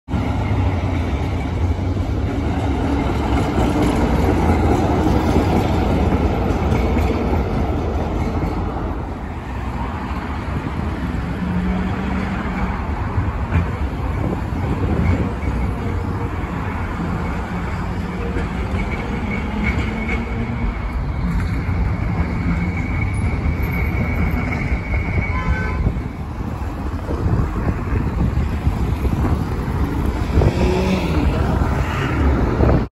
Трамваи и чемпионат Европы по футболу/Trams and UEFA Cup
В Санкт-Петербурге, как и в некоторых други городах Европы, 11 июня – 11 июля проводятся матчи чемпионата Европы по футболу. И трамваи украшены к этому событию.  Надеюсь это не краска, а плёнка и её уберут после окончания чемпионата.

In St. Petersburg, wie auch in einigen anderen europäischen Städten, finden vom 11. Juni bis 11. Juli Spiele der Fußball-Europameisterschaft statt.  Die Straßenbahnen haben für diese Veranstaltung ihre Farbe gewechselt.